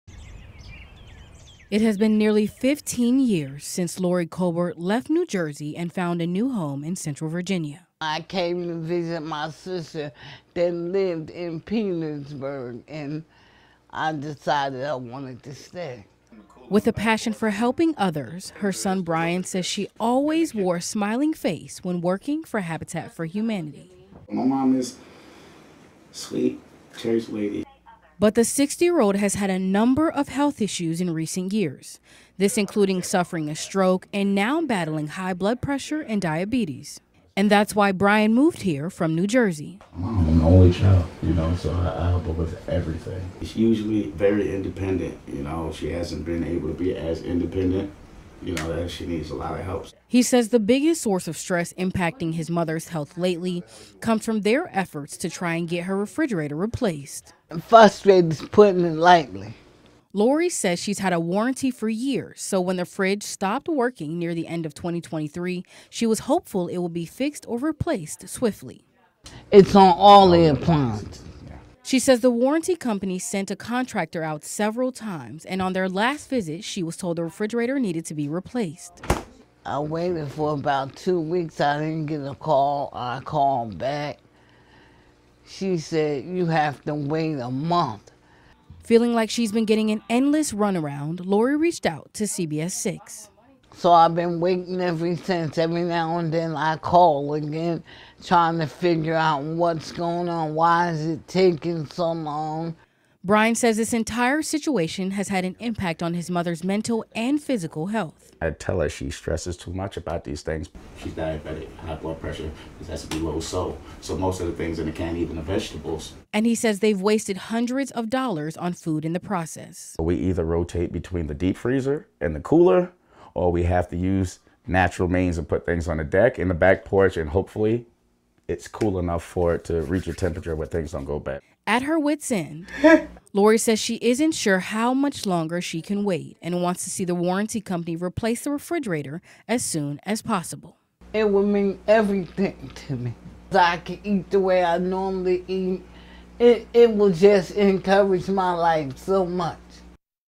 Home warranty company frustrates Virginia woman living without a working fridge
Lori Colbert, 60, called CBS 6 for help because her home warranty company has not been able to replace her broken refrigerator.